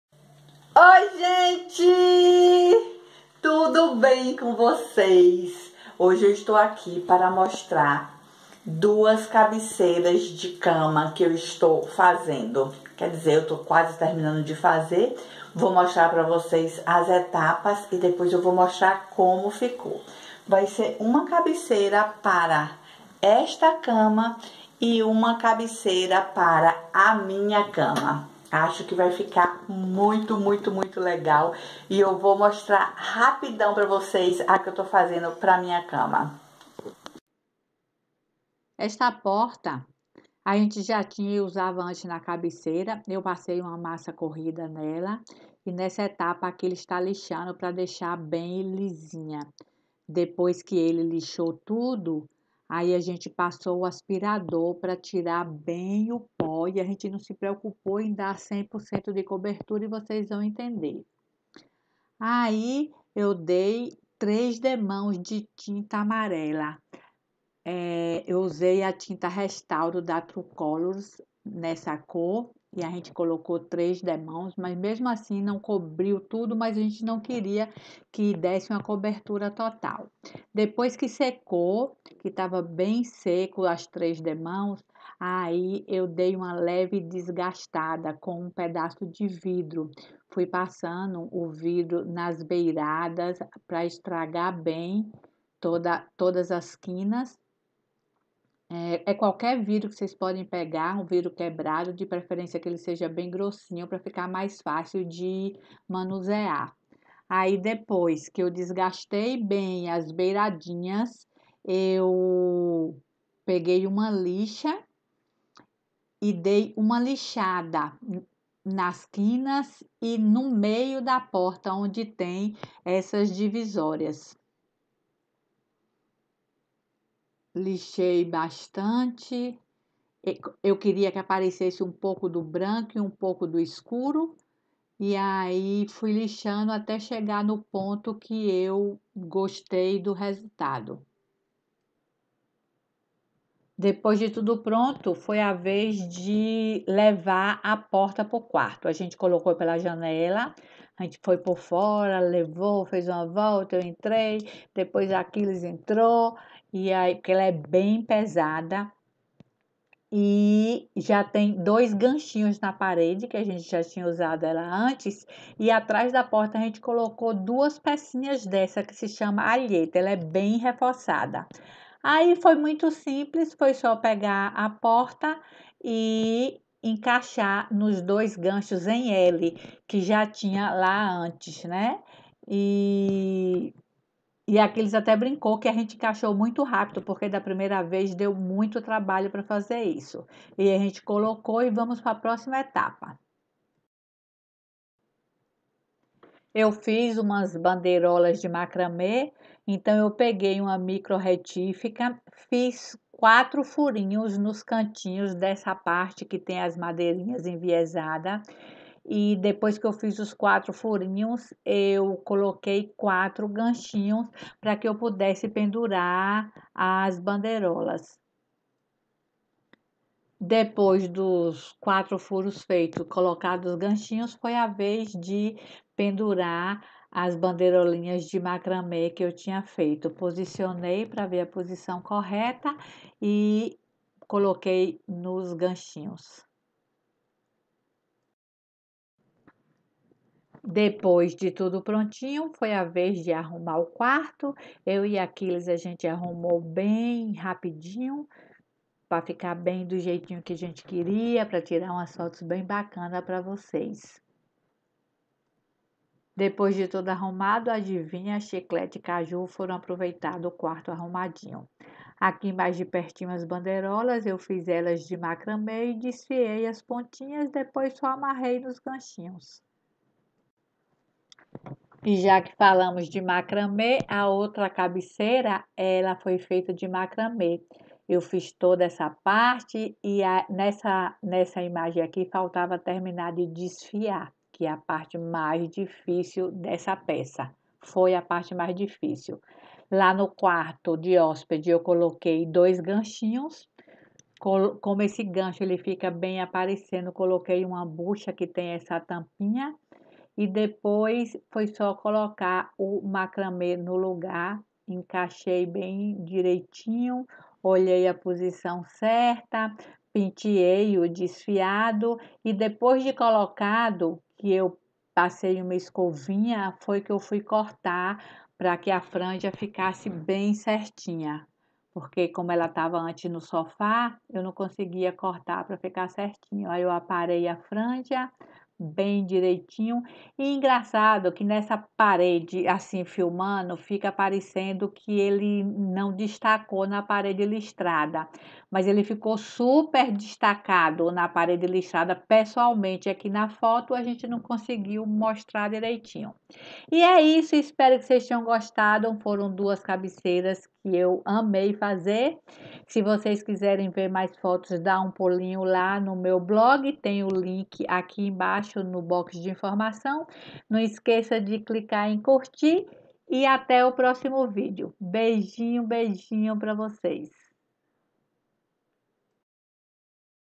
Ideia para duas cabeceiras de cama - Uma com porta e uma com macramé.
Duas cabeceiras de cama. Uma foi feita com uma porta e a outra com uma painel em macramé.